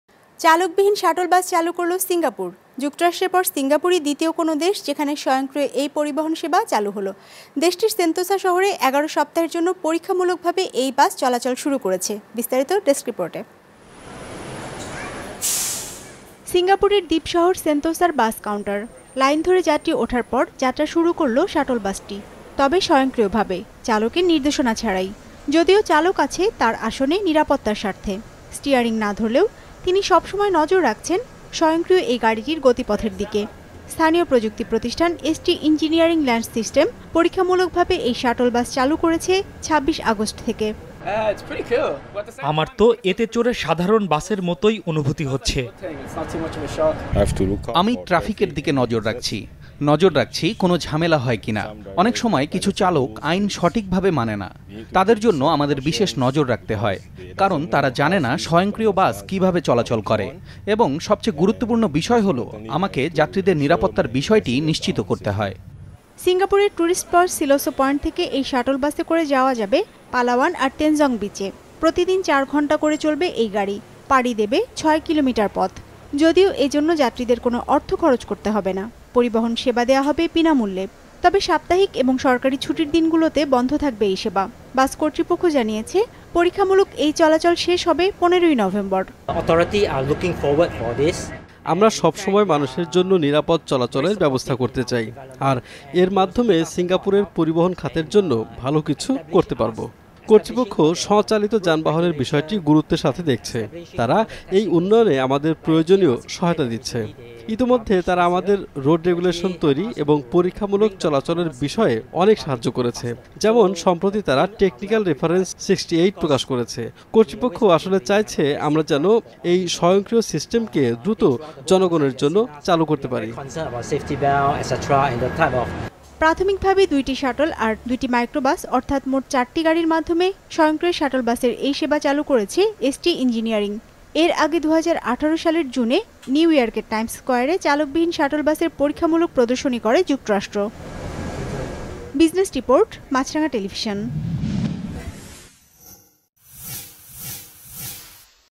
সিঙ্গাপুরে চালকবিহীন শাটল বাস...
চালকবিহীন শাটল বাস চালু করলো সিংগাপুর। যুক্তরাষ্ট্রের পর সিঙ্গাপুরই দ্বিতীয় কোনো দেশ যেখানে স্বয়ক্রিয় এই পরিবহন সেবা শুরু হলো। দেশটির সেন্তোসা শহরে ১১ সপ্তাহের জন্য পরীক্ষামূলক ভাবে এই বাস চলাচল শুরু করেছে...